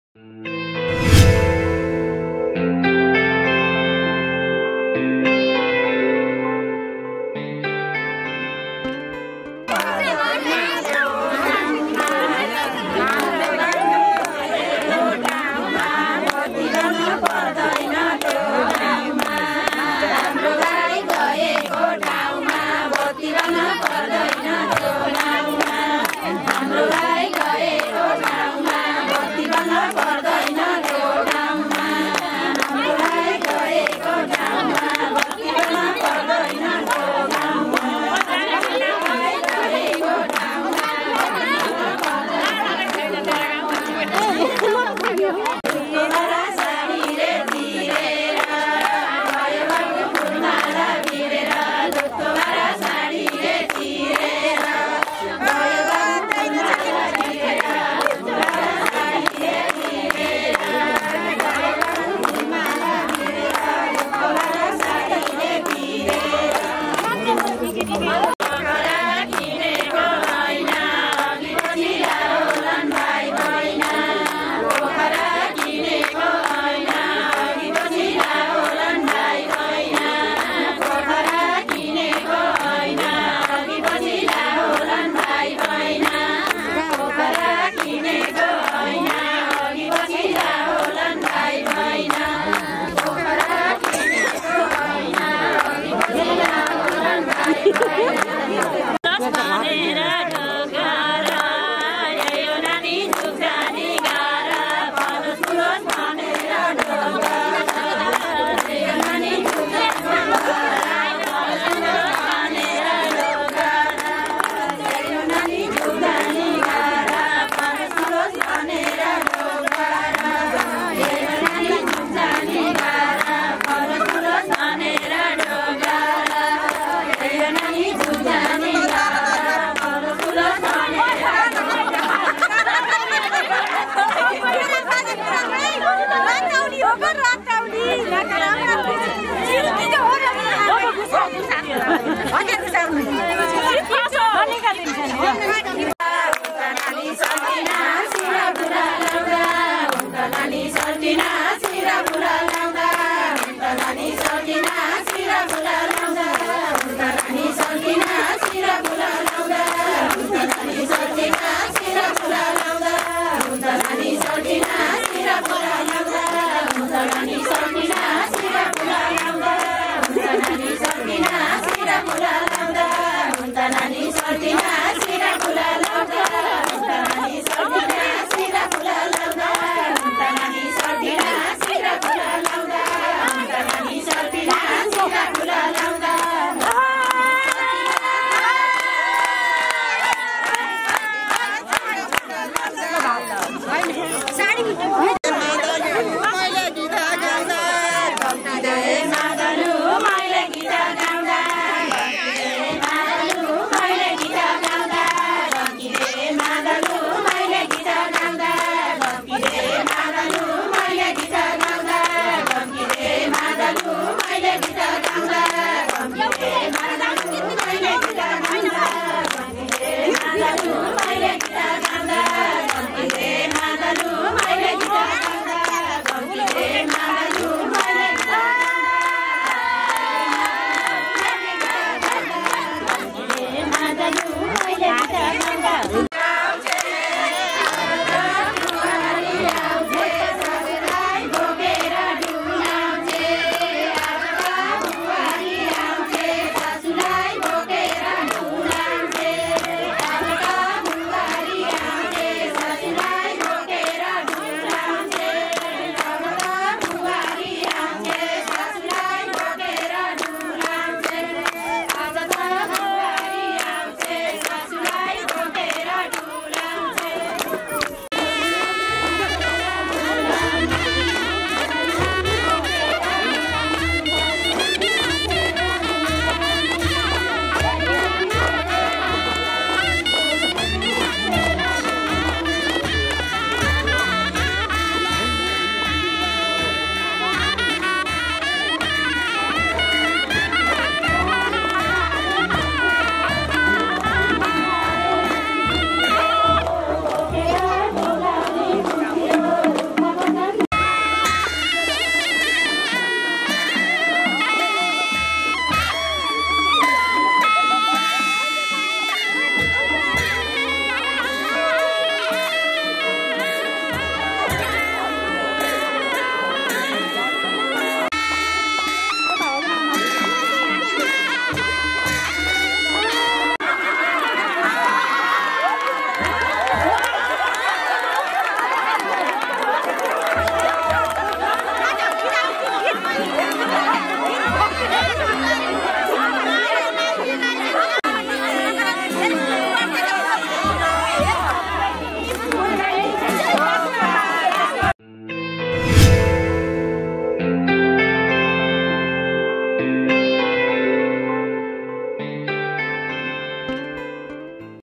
Nepali Wedding II रत्यौली नाच्दा के के देख्न र सुन्न पाईन्छ II Surash & Manita
रत्यौली नाच्दा के के देख्न र सुन्न पाईन्छ । यसको सुरुवात कहिले कसरी भयो जान्न साह्रै मन लाग्यो । अनि रत्यौलीमा महिलाहरु ले मात्र किन आफु अनुकुल गितहरु गाउँछन । पुरुष हरुको सहभागीता किन रत्यौली मा नगराउने । भिडियो हेरी राम्रो प्रतिक्रिया र सुझाव को प्रतिक्षामा रहनेछु ।
II Thapa Digital Photo & Video Graphi II